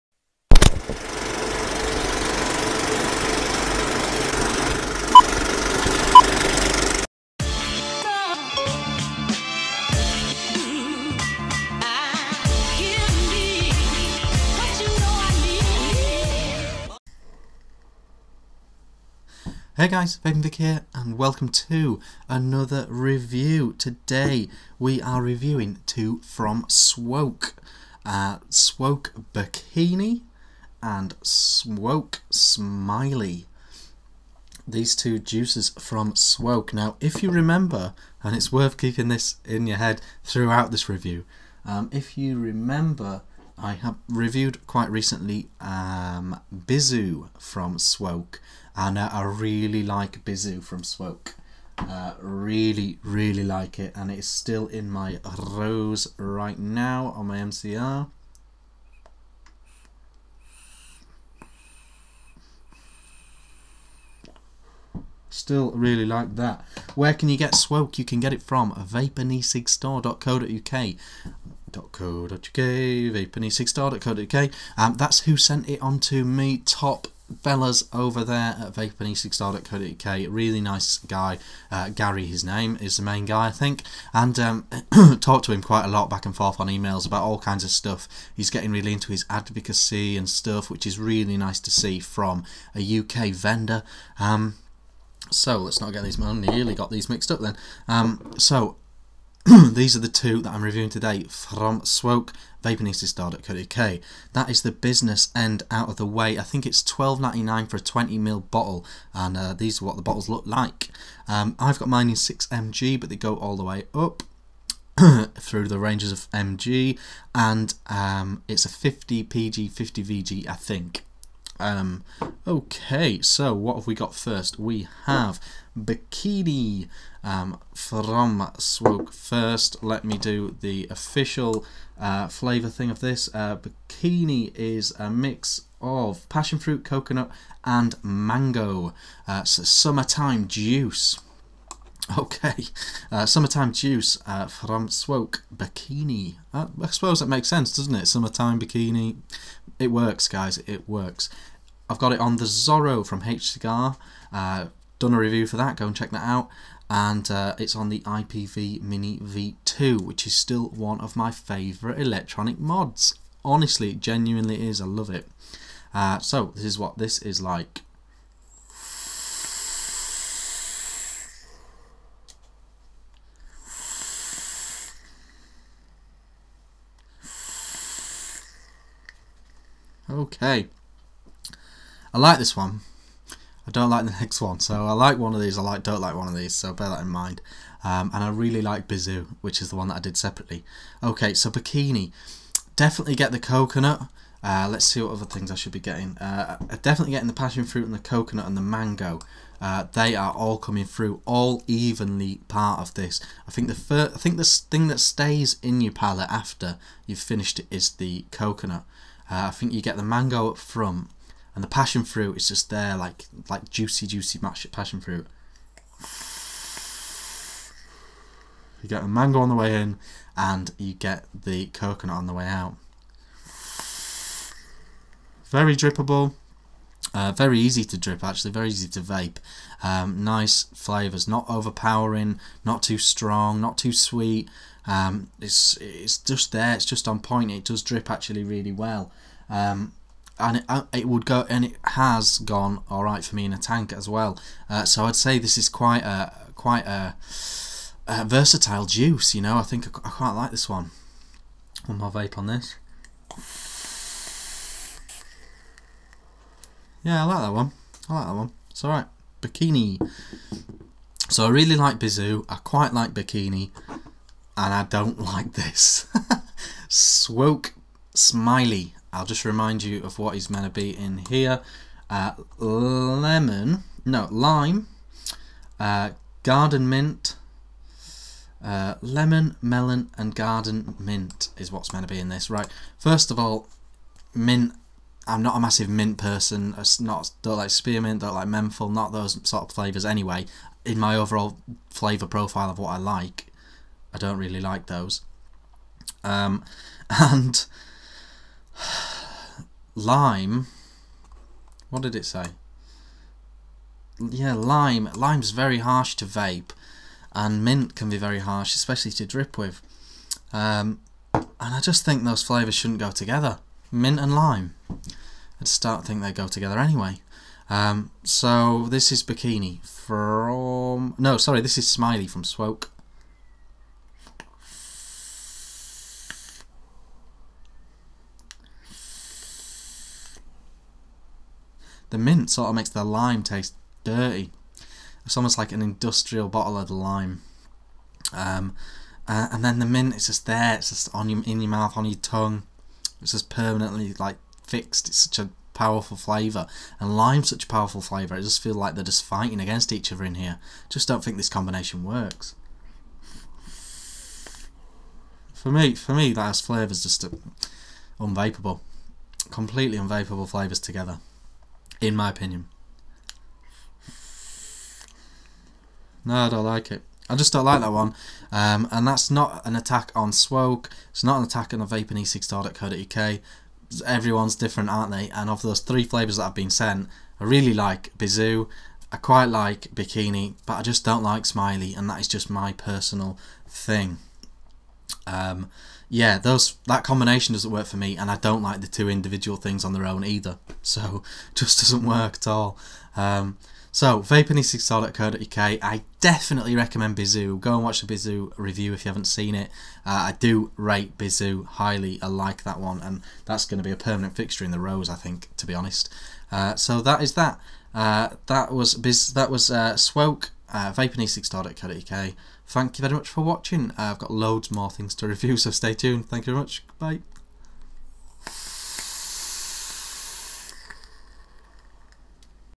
Bikini & Smiley from Swoke - Juice Review
Hey guys this is my review for 2 from swoke. One I like and one I really don't :)

Direct Link: vapeandecigstore.co.uk/SwokePremiumELiquid/cid1258/SwokePremiumELiquid.asp